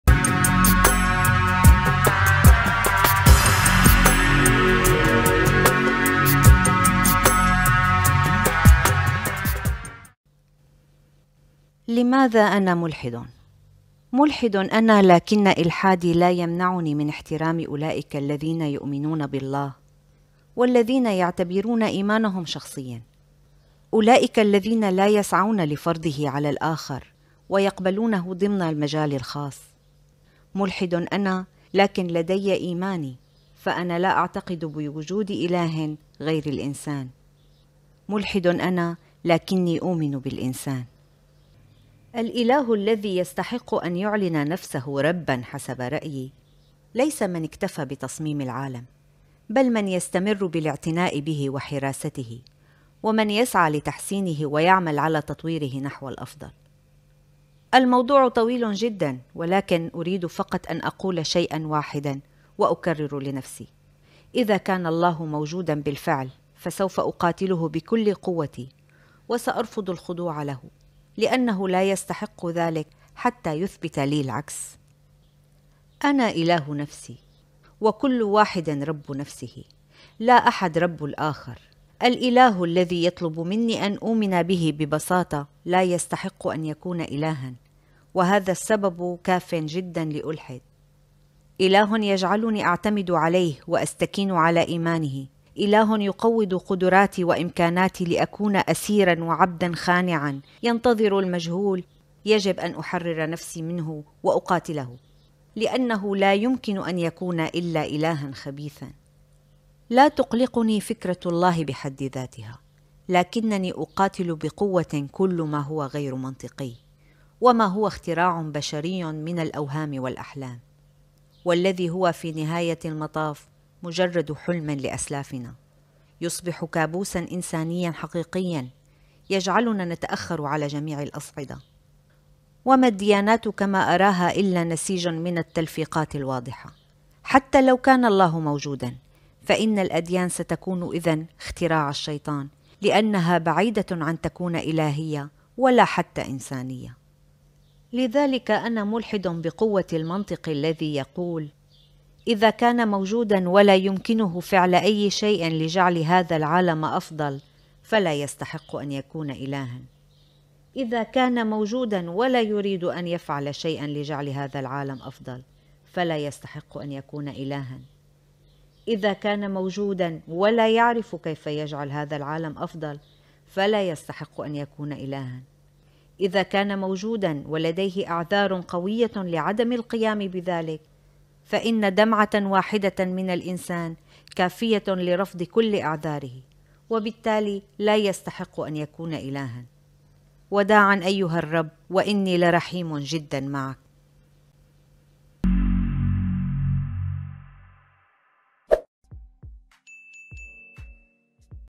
336 - قناة تعلموا دين ربكم - لماذا انا ملحد ؟
مقال على موقعي الرسمي
ملحدٌ أنا لكن إلحادي لا يمنعني من احترام أولئك الذين يؤمنون بالله والذين يعتبرون إيمانهم شخصيًا، أولائك الذين لا يسعون لفرضه على الآخر ويقبلونه ضمن المجال الخاص. ملحدٌ أنا لكن لدي إيماني فأنا لا أعتقد بوجود آلهٍ غير الإنسان. ملحدٌ أنا لكني أومن بالانسان.

الإله الذي يستحق أن يعلن نفسه رب حسب رأيي، ليس من اكتفى بتصميم العالم، بل من يستمر باعتناء به وحراسته، من يسعى لتحسينه ويعمل على تطويره نحو الأفضل. الموضوع طويل جداً ولكن أريد فقط أن أقول شيئًا واحدًا وأكرر لنفسي: "إذا كان الله موجودًا بالفعل، فسوف أقاتله بكل قوتي وسأرفض الخضوع له لأنه لا يستحق ذلك حتى يثبت لي العكس".
أنا إله نفسي. وكل واحد رب نفسه. لا أحد رب الآخر. الإله الذي يطلب مني أن أؤمن به ببساطة لا يستحق أن يكون إلهاً وهذا السبب كافٍ جداً لألحد. إلهٌ يجعلني أعتمد عليه وأستكين على إيمانه، إله يقوض قدراتي وإمكاناتي لأكون أسيراً وعبداً خانعا ينتظر المجهول يجب أن أحرر نفسي منه وأقاتله. لأنه لا يمكن أن يكون إلّا إلهً خبيث.
لا تقلقني فكرة الله بحد ذاتها، لكنني أقاتل بقوة كل ما هو غير منطقي، وما هو اختراع بشري من الأوهام والأحلام، والذي هو في نهاية المطاف مجرد حلم لأسلافنا، يصبح كابوسًا إنسانيًا حقيقيًا يجعلنا نتأخر على جميع الأصعدة. وما الديانات كما أراها إلّا نسيج من التلفيقات الواضحة. حتى لو كان الله موجودًا، فإن الأديان ستكون إذن اختراع الشيطان لأنها بعيدة عن أن تكون إلهية ولا حتى إنسانية.
لذلك أنا ملحد بقوة المنطق الّذي يقول:
 إذا كان موجودًا ولا يمكنه فعل أي شيء لجعل هذا العالم أفضل، فلا يستحق أن يكون إلهاً.
 إذا كان موجودًا ولا يريد أن يفعل شيئًا لجعل هذا العالم أفضل، فلا يستحق أن يكون إلهاً.
 إذا كان موجودًا ولا يعرف كيف يجعل هذا العالم أفضل، فلا يستحق أن يكون إلهاً.
 إذا كان موجودًا ولديه أعذار قوية لعدم القيام بذلك، فأن دمعة واحدة من الإنسان كافية لرفض كل أعذاره وبالتالي لا يتستحق أن تكون الله.
وداعاً أيها الرّب وأنّي لرحيم جداً معك.
00:00
2:00
3:00